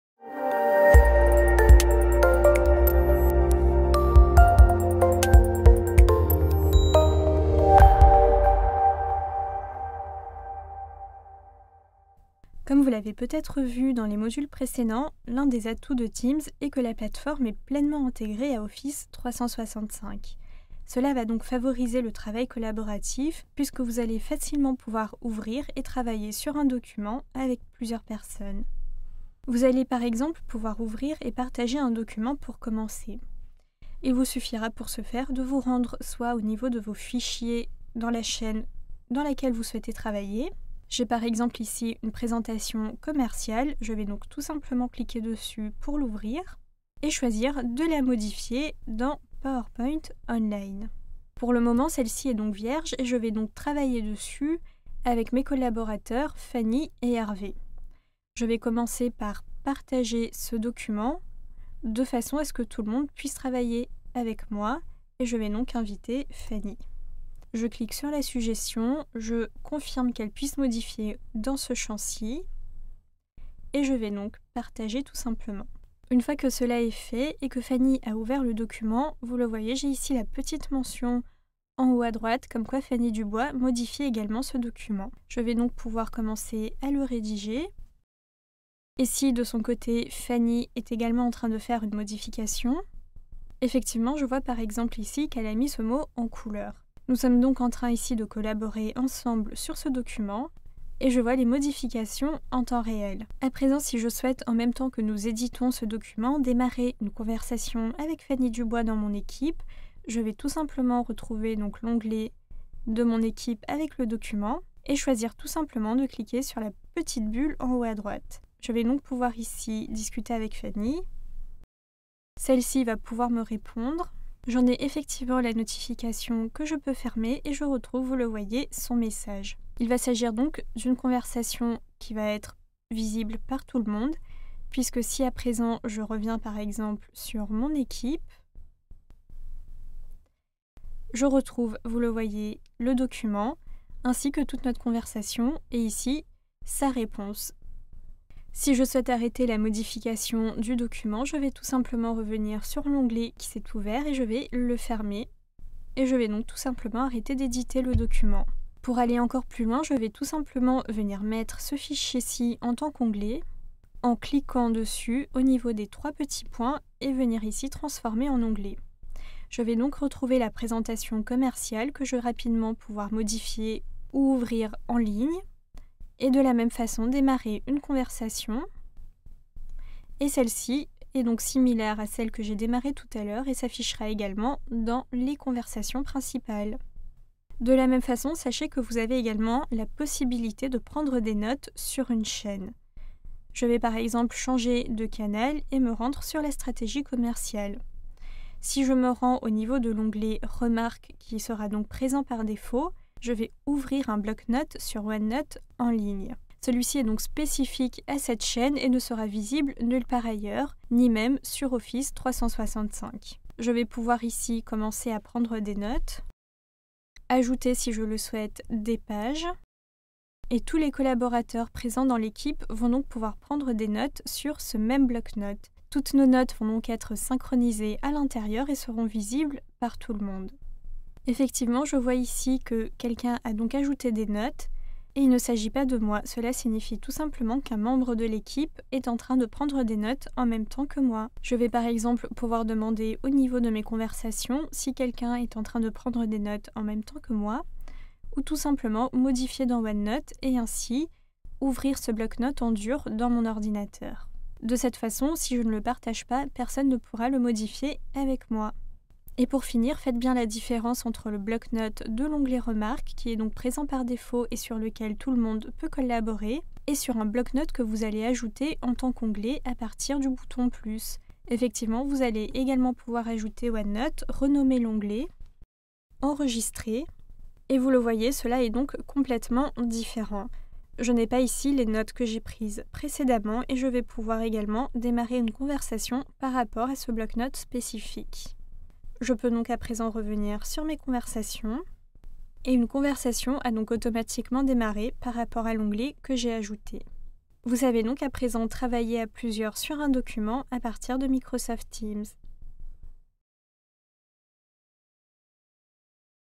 Travailler à plusieurs sur un document
L'un des atouts de Teams est que la plateforme est pleinement intégrée à Office 365. Cela favorise donc le travail collaboratif, puisque vous pouvez facilement ouvrir et traiter un document avec l'application qui lui correspond. Allez plus loin en partageant le fichier, de façon à voir les modifications faites par vos collègues sur ce fichier partagé.